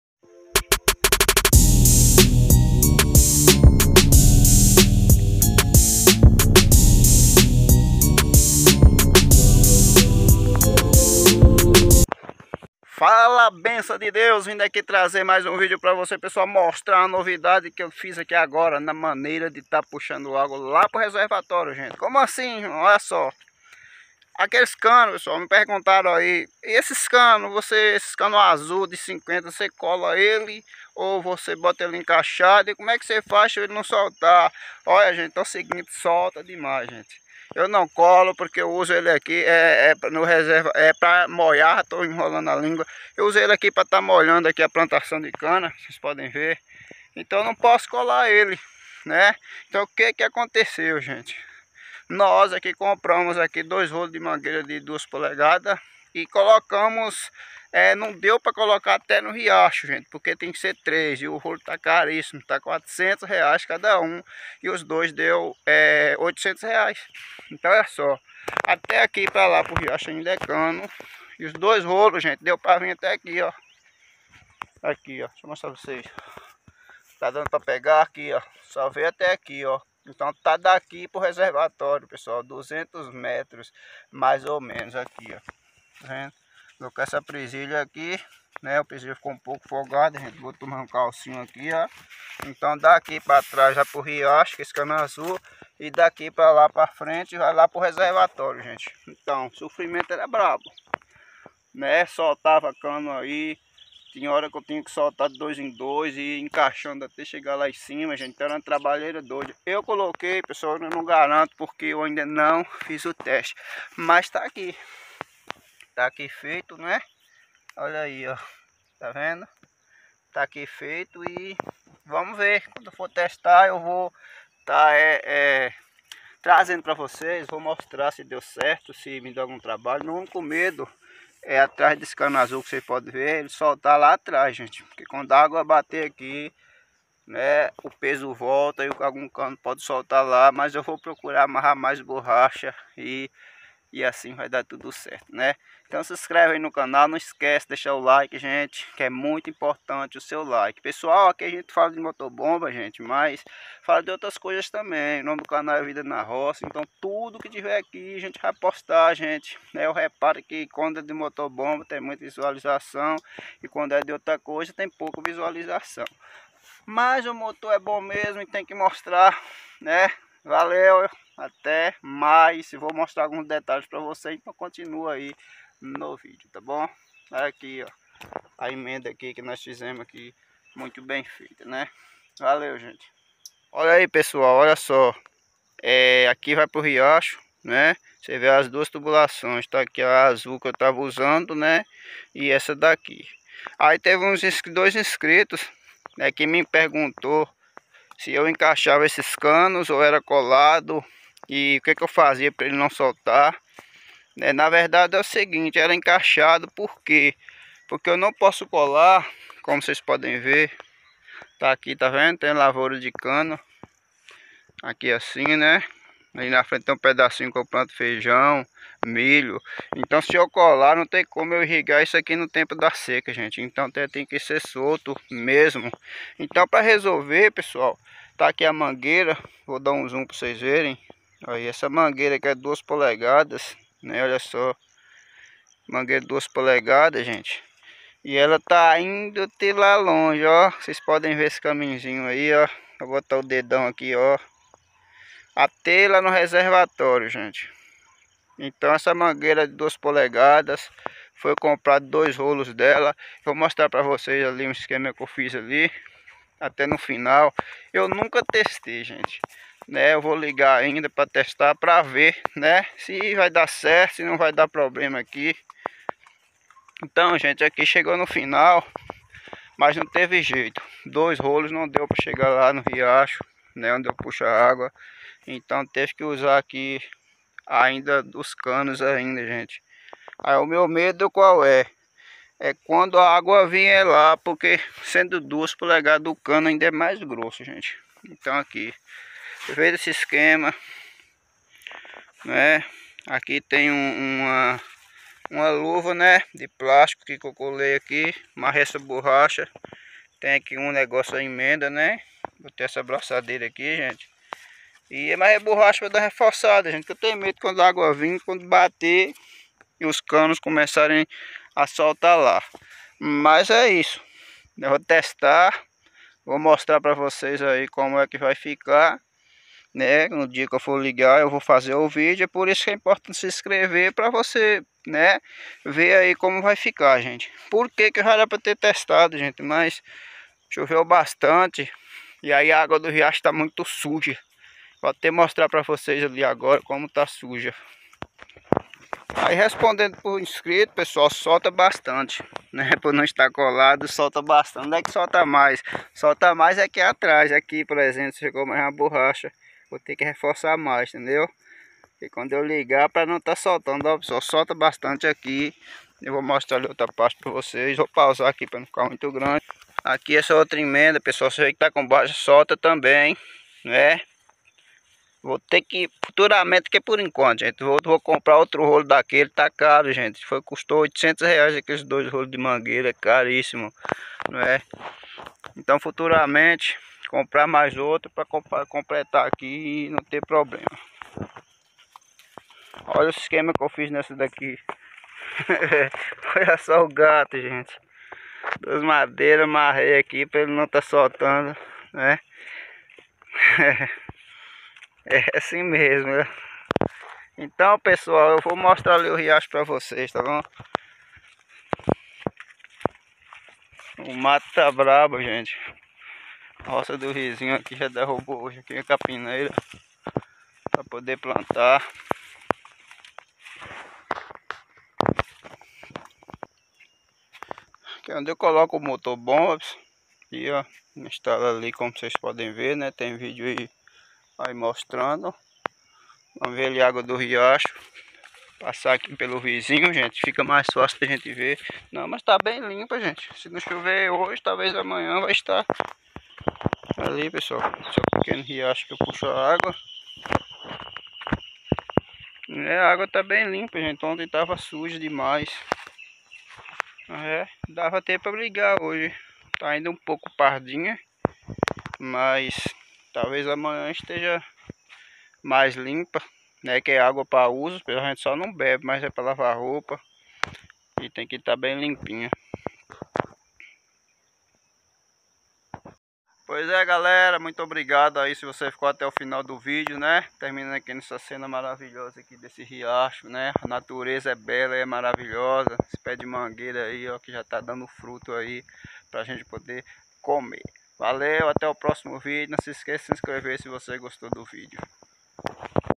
Agora não solta mais ..Nova tubulação do Motobomba Branco..
agora troquei a tubulação do Motobomba..